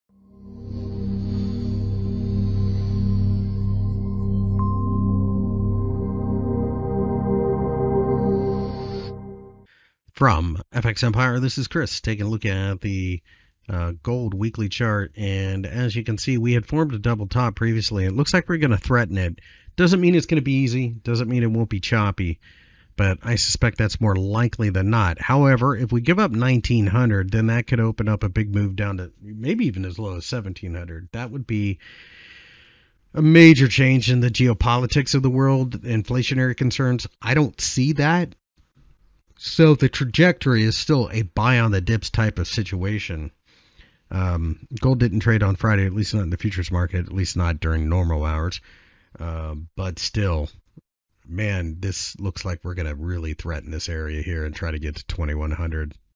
Gold Technical Analysis for the Week of April 18, 2022 by FXEmpire
Gold markets have rallied a bit during the course of the week to break above the $1970 area. Because of this, it looks like we are reaching to challenge the previous double top.

Find more information about Forex and Commodities News Technical Analysis Fundamental Analysis and Brokers on FX Empire